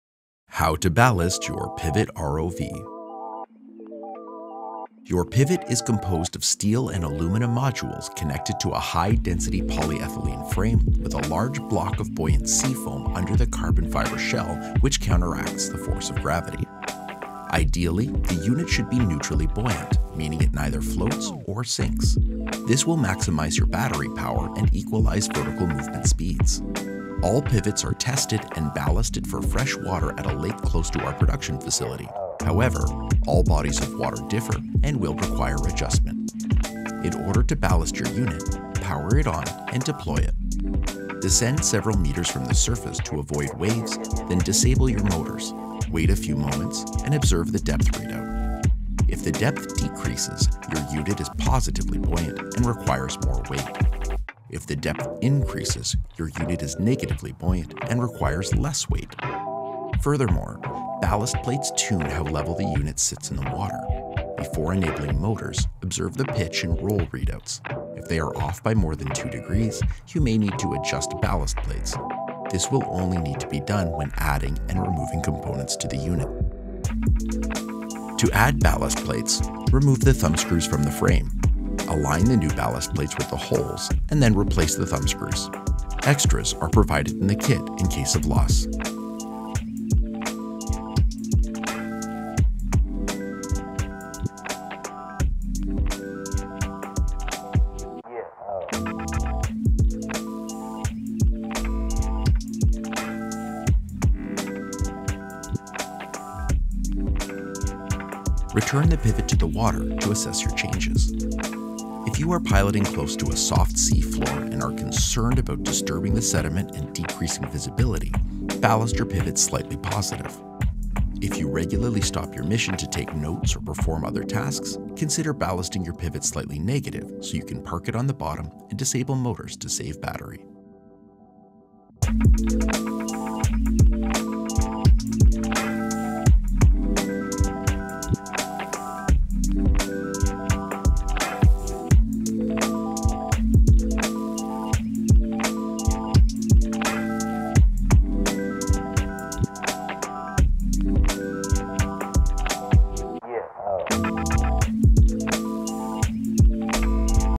Ballasting the PIVOT ROV
Depending on the situation or underwater environment, ballasting can vary. Deep Trekker's PIVOT ROV can quickly and easily be adjusted to for positive, neutral, or negative buoyancy to simplify a number of situations. This video acts as a visual walkthrough to help you be deployment ready in minutes.

0:00 Intro
0:05 Maintaining Neutral Buoyancy
1:17 Adjusting Ballast Plates
2:12 Outro